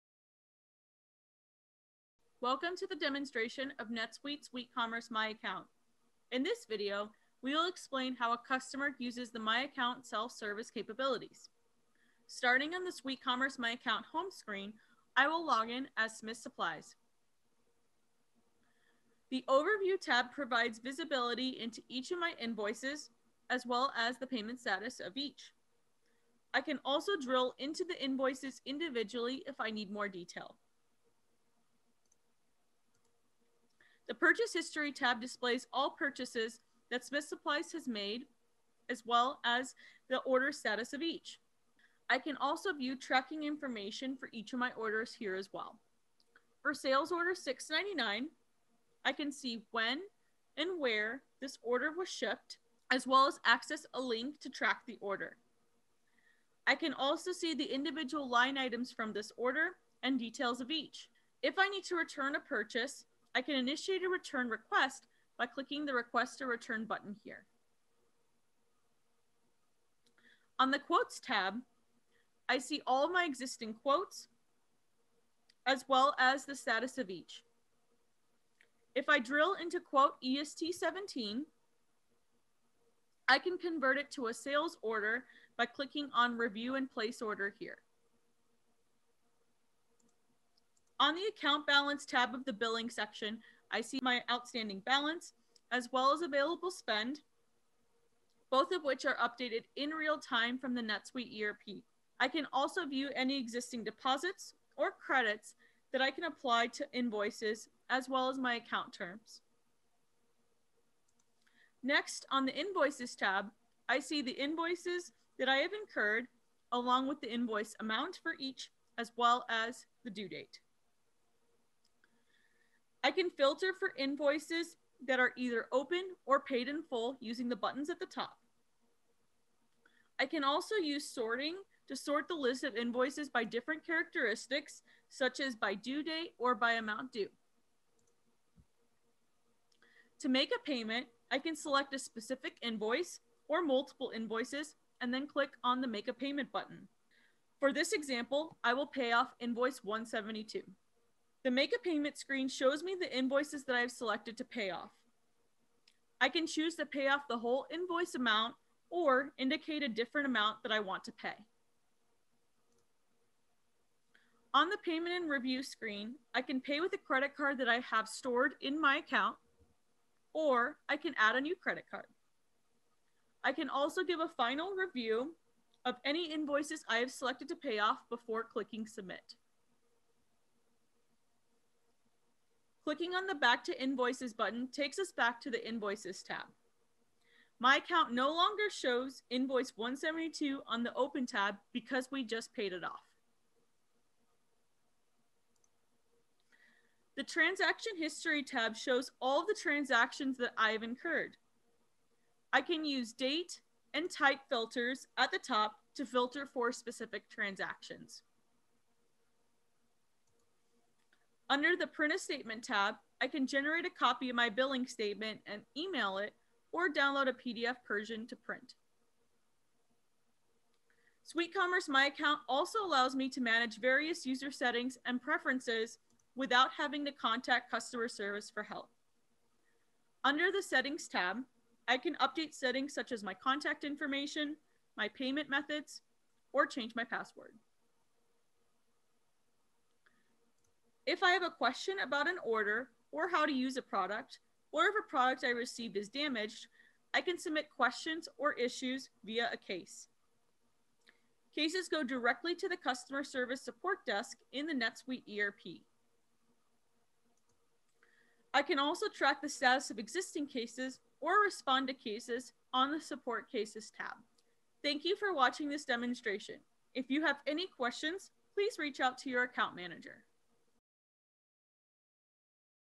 NetSuite SuiteCommerce MyAccount: Customers
NetSuite SuiteCommerce MyAccount is a B2B self-service online portal where your customers can manage their account information, view recent transactions, pay invoices, manage cases and convert quotes to orders. In this demonstration, we'll explain how to use NetSuite SuiteCommerce MyAccount from a customer's perspective. We'll start by looking into an existing sales order and explain how to track individual orders as well as initiate and track a return request. We'll then explain how to manage outstanding balances, deposits or credits, and we'll view invoice details and make payments—all within SuiteCommerce MyAccount.

Liked this video? Check out this!